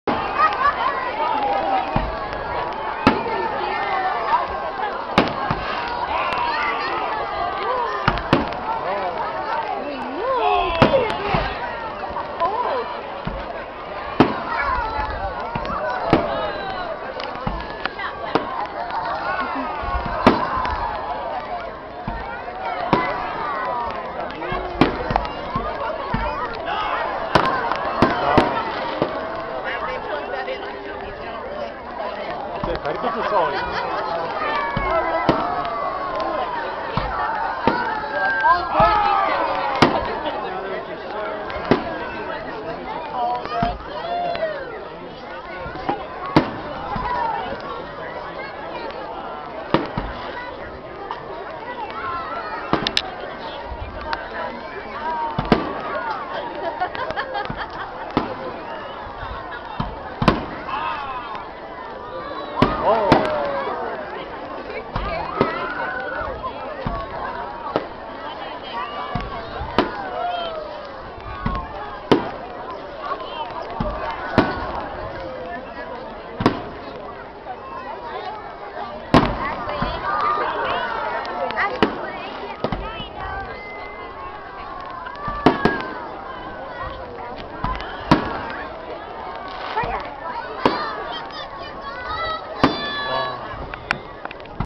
4th of July San Fran
We went to the marina in berkeley to watch the fireworks-gas craic!